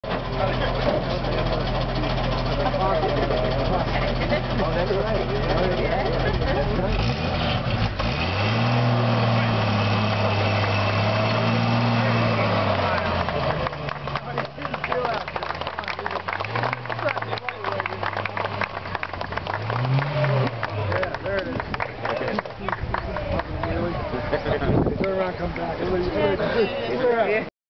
Beaverkill Covered Bridge reopens
The Beaverkill Covered Bridge - originally built circa 1865 - reopens 6/21/17 after extensive renovation and repair.